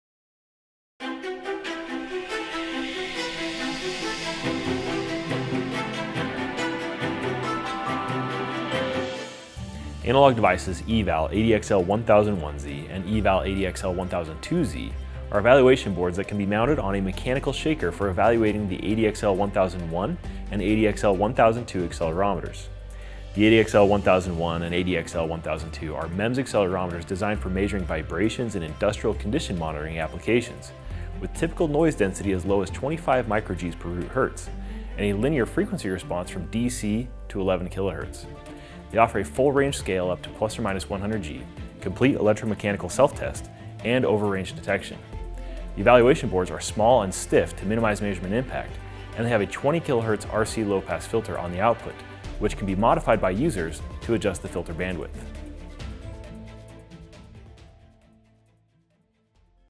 Analog Devices EVAL-ADXL1001Z/1002Z Evaluation Boards | New Product Brief
Analog Devices’ EVAL-ADXL1001Z and EVAL-ADXL1002Z are evaluation boards that can be mounted on a mechanical shaker for evaluating the ADXL1001 and ADXL1002 accelerometers. The ADXL1001 and ADXL1002 are MEMS accelerometers designed for measuring vibrations in industrial-condition monitoring applications, with typical noise density as low as 25 μg/√Hz and a linear frequency response from dc to 11 kHz. They offer a full-scale range up to ±100g, complete electromechanical self-test, and over range detection. The evaluation boards are small and stiff to minimize measurement impact, and they have a 20 kHz RC low-pass filter on the output, which can be modified by users to adjust the filter bandwidth.

Don't have AAC account yet? Register here for free, and always will be.